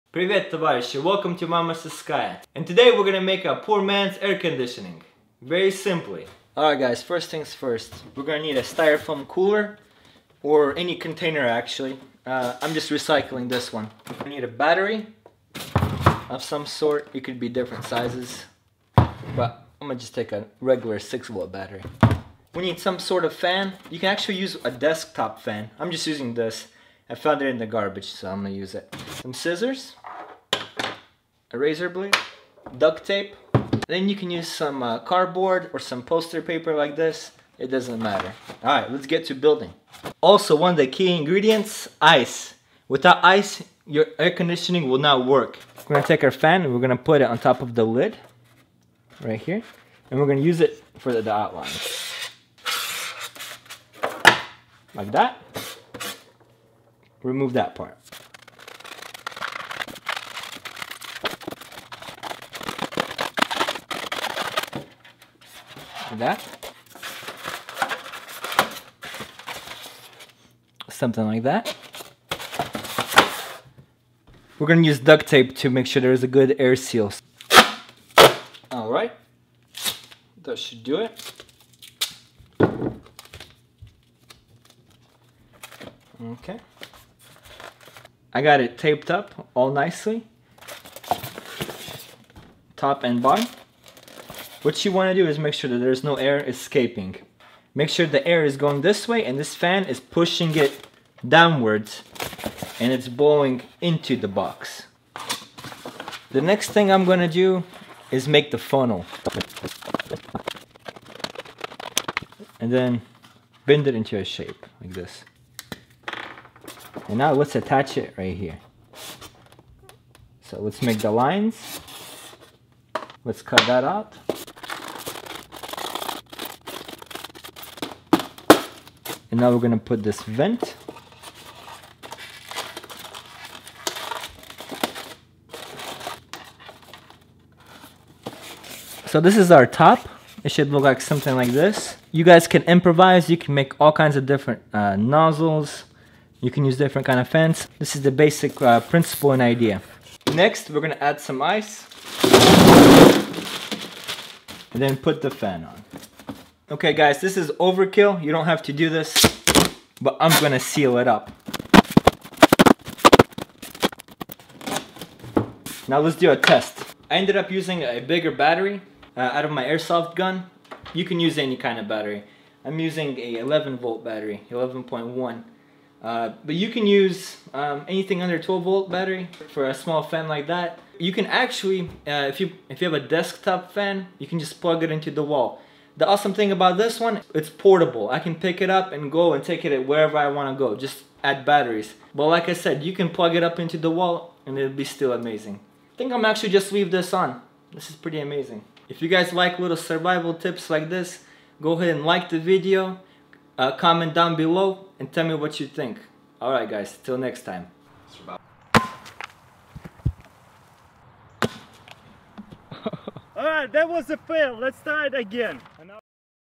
Poor Man's Air Conditioning
Fun way to make your room cooler this summer ,If you don't have AC than this is a great solution .  All you need is a box ,Ice,a fan,tape and a battery to power the fan .Enjoy!!